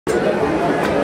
SHIV SEWA MOMENT OF AMARNATH YATRA
Amarnath Yatra is one of the most famous hindu cave shrine dedicated to the Lord Shiva located at the height of 14000 feet on the Amar Nath Mountain about 144 kms east to Srinagar in state Jammu & Kashmir. The shrine is said to be 5,000 yrs old. Lord Amarnath is in the form of an ice Shiva-Lingam.